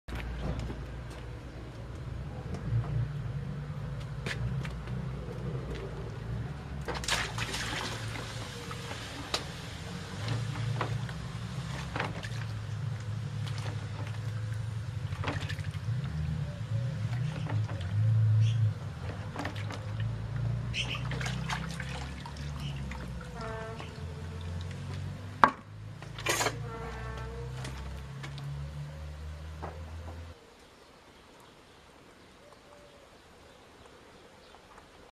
A whiskey barrel DIY. The firing process a wooden barrel...
How to make a whiskey barrel at home. The  firing process a wooden barrel...